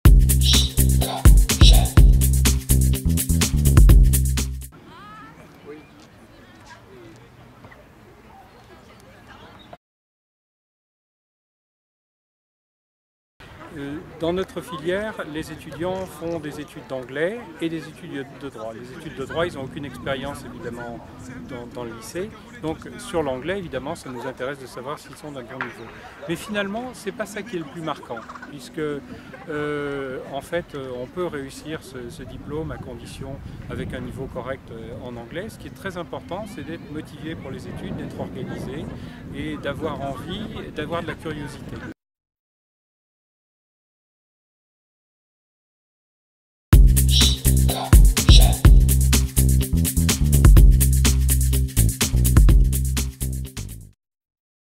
La double licence droit anglais : mieux vaut être motivé et organisé
Pour réussir un double diplôme comme le double diplôme droit anglais de Saint Quentin en Yvelines, capacité de travail et organisation sont nécessaires.

La double licence droit anglais : l' éco, l'histoire-géo, la philo, tout compte.